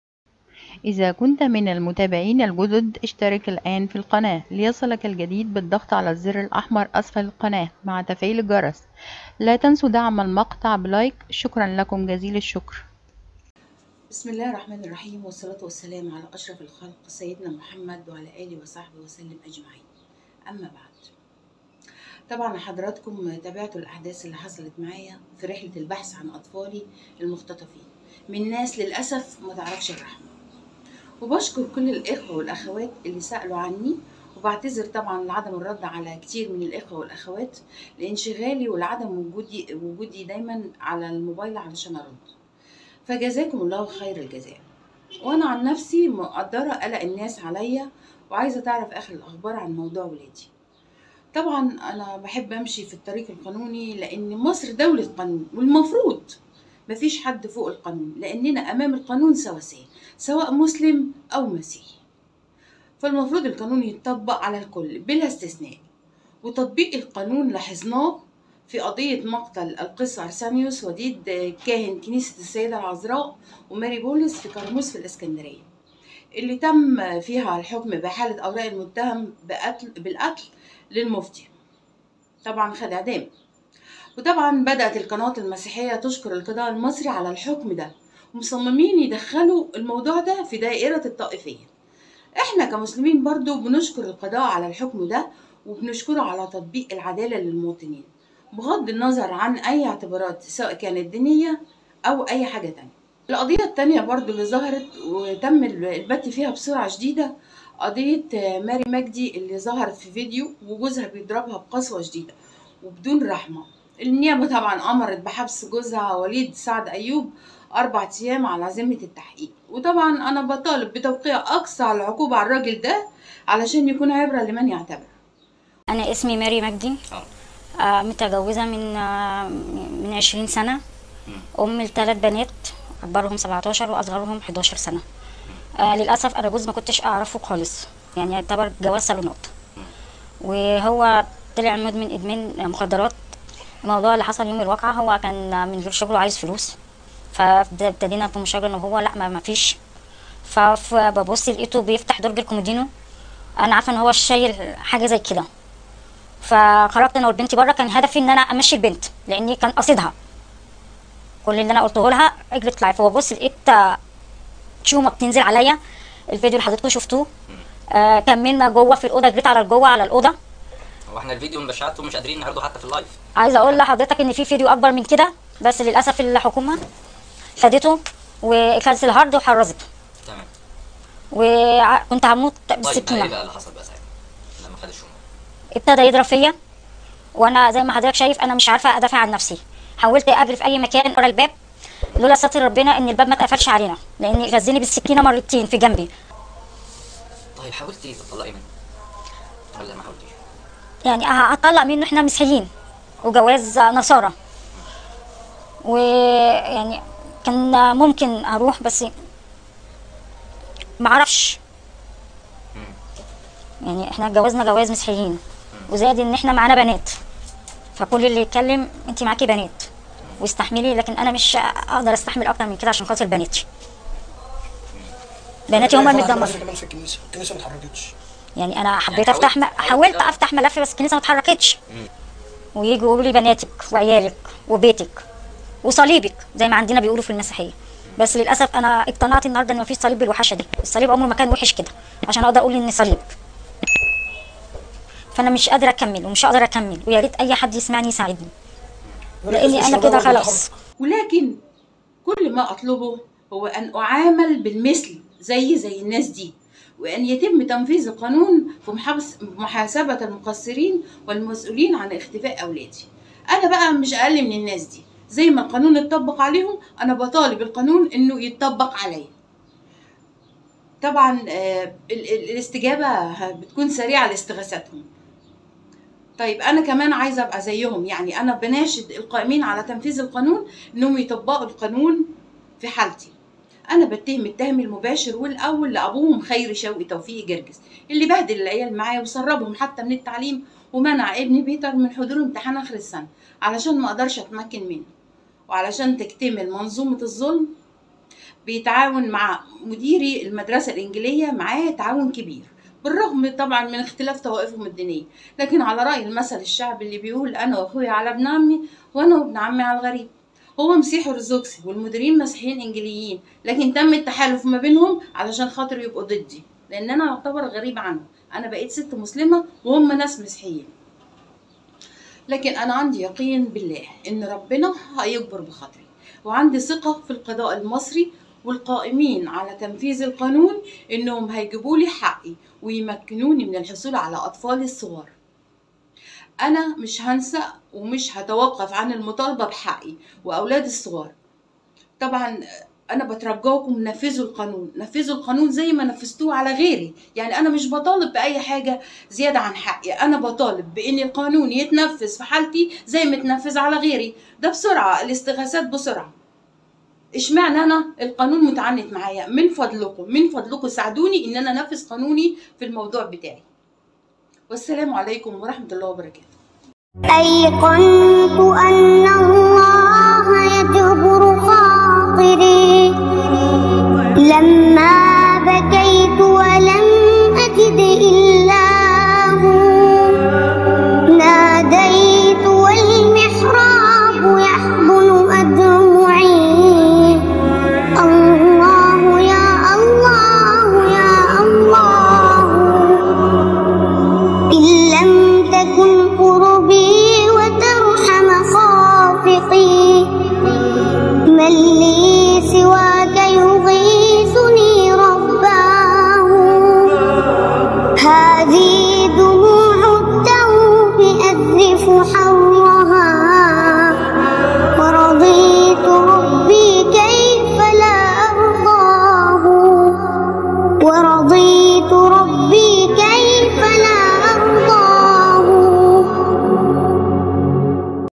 تطبيق القانون بين مقتل القس أرسانيوس وضرب ماري مجدي وأبناء نهي السيد
رابط الباي بال الخاص بالقناة